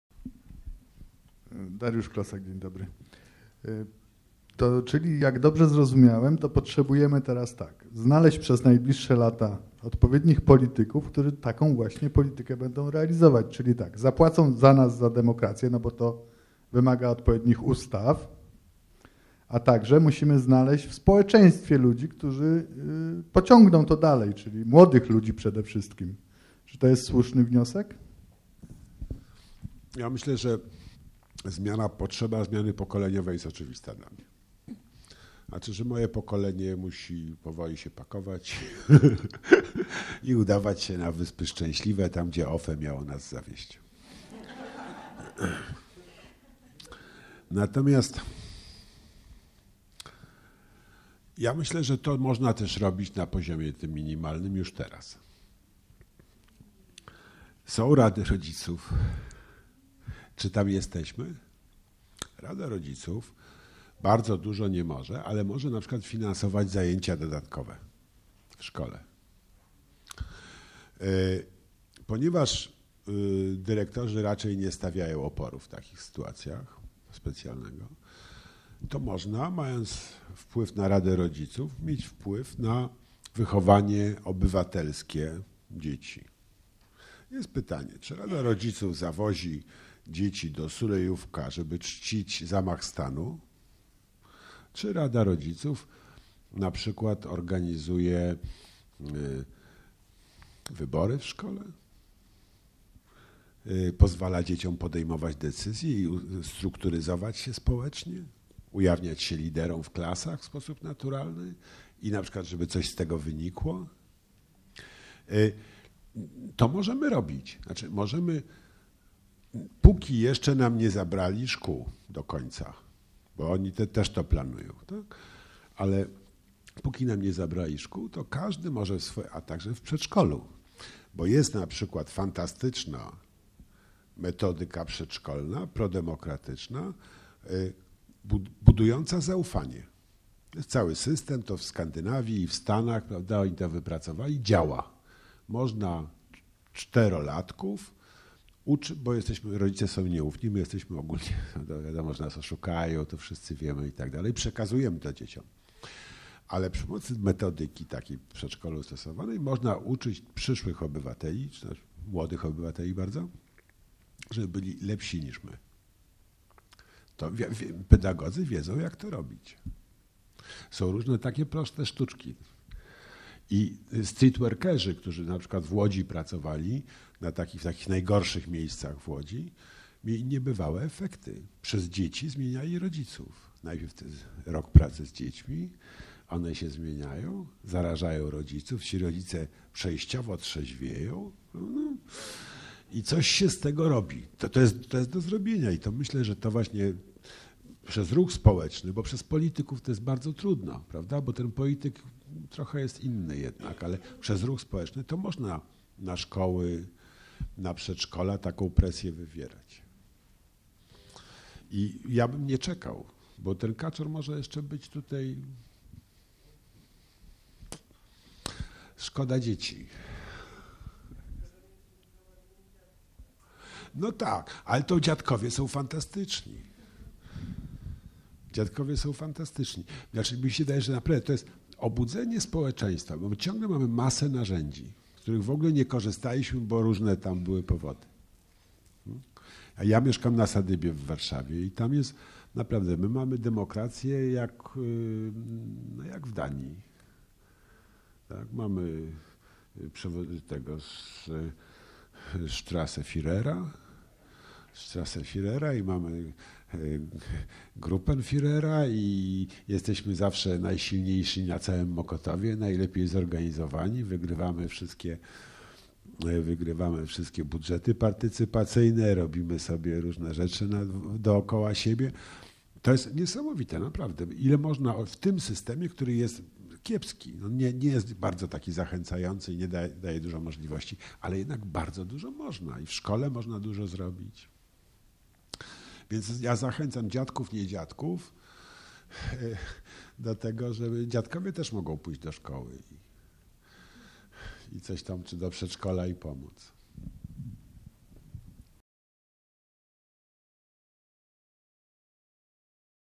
Jacek Żakowski o wychowaniu młodego pokolenia
W dniu 26. kwietnia 2016 roku odbyło się spotkanie z redaktorem Jackiem Żakowskim, organizowane przez Grupę KOD Praga Południe. Podczas spotkania popularny dziennikarz mówił o sytuacji społeczno-politycznej w kraju, o miejscu Komitetu Obrony Demokracji w procesie uwrażliwiania społeczeństwa na takie wartości jak demokracja i praworządność, a także o potrzebie zmian w polityce, jaką obserwujemy od kilku lat w Polsce.